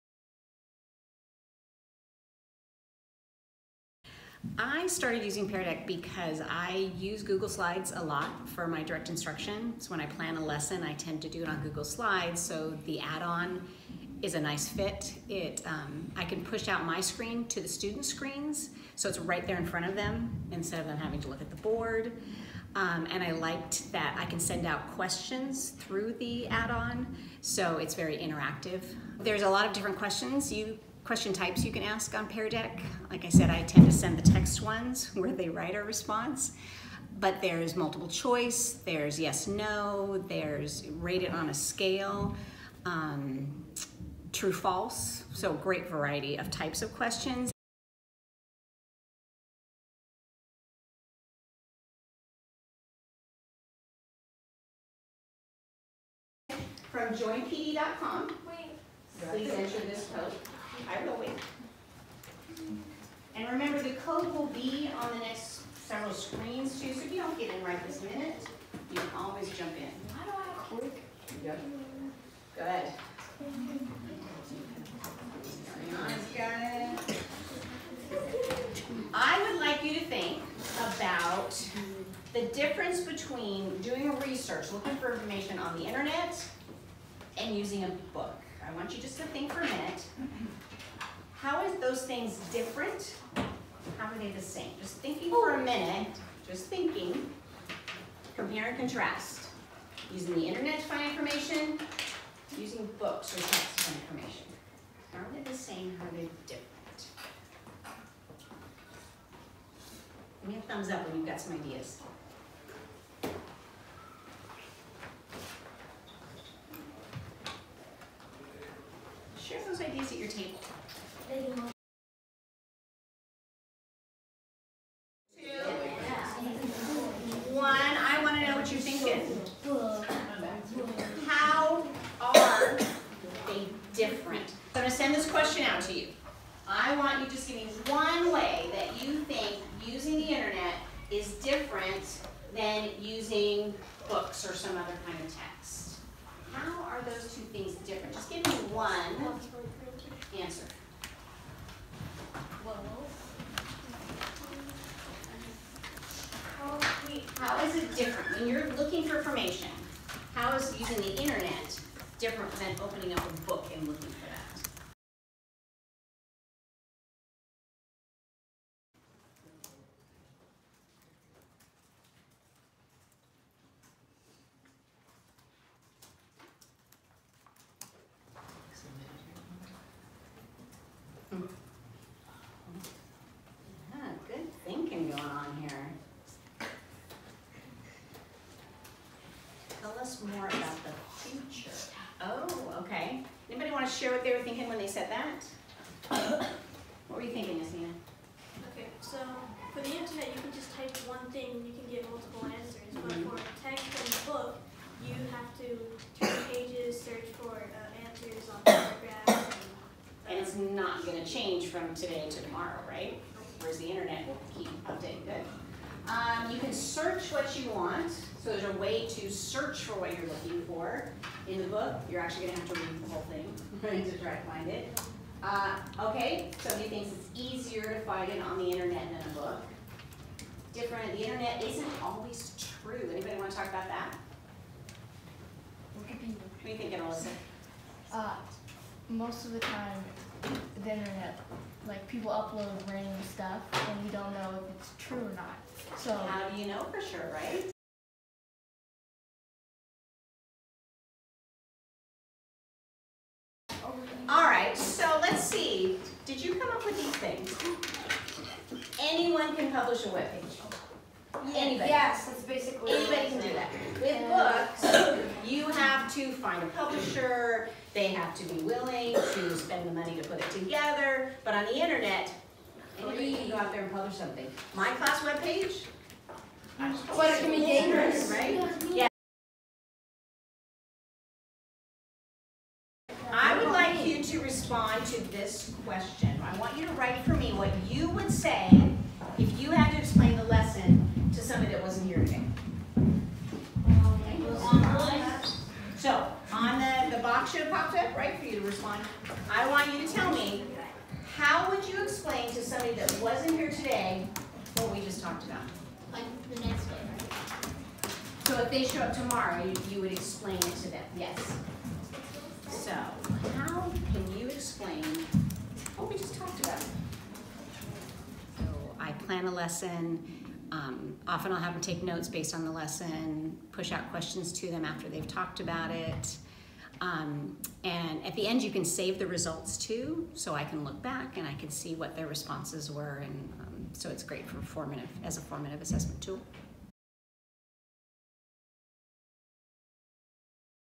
Using Pear Deck for Formative Assessment
Watch this teacher explain how she uses Pear Deck to guide lessons, engage in discussions and assess her students.